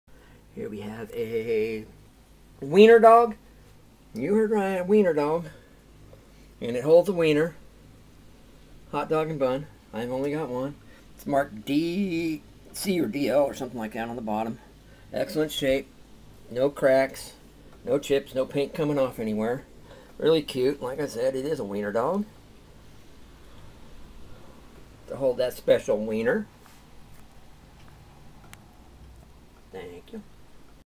Unique Vintage Retro 1950's Ceramic HotDog Hot Dog Holder VGC bottom marked DL
For sale a used ceramic hot dog holder. This hot dog holder is from the 1950's and is ceramic. Hot dog holder is in great shape with no chips, cracks. Holder is of a Dachshund dog and measures about 10 1/2 inches (26.67 cm) long, about 3 inches (7.62 cm) wide and about 2 1/2 inches (6.35 cm) at the tallest point. This item is for sale.